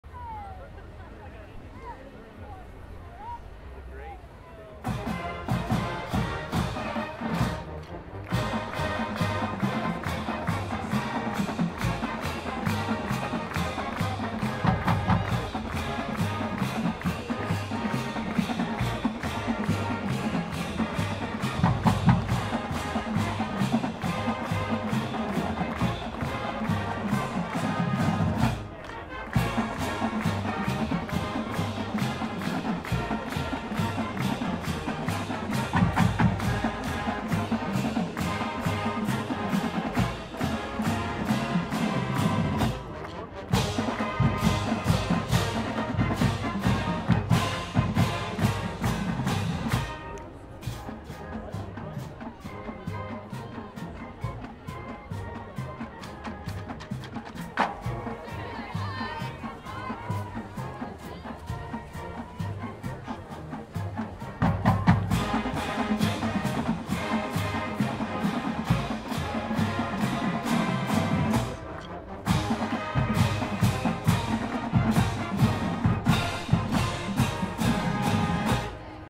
KU Marching Jayhawks playing Kansas form the Hill Senior Day 11-27-2021
KU Marching Jayhawks playing Kansas